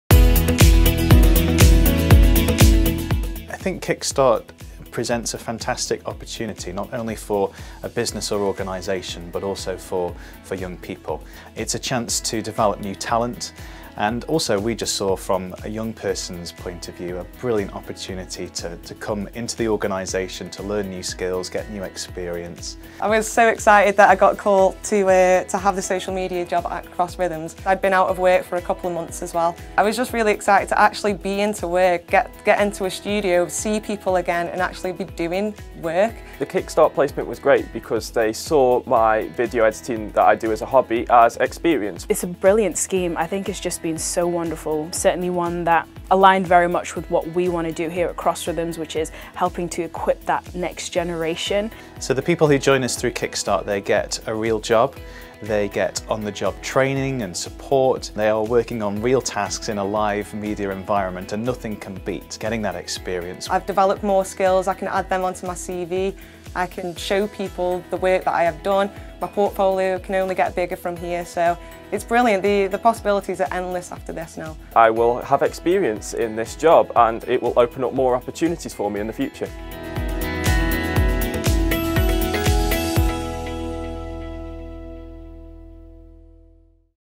Kickstarting at Cross Rhythms radio
"I think Kickstart represents a fantastic opportunity for not just a business but also young people.'' That's what Cross Rhythms Radio says about their new Kickstarter.

Want to find out more about the Kickstart programme as a business? , and if you're a young person looking for a placement, go to the hashtag KickStartinStaffs every Thursday 3pm to 5pm for the latest opportunities.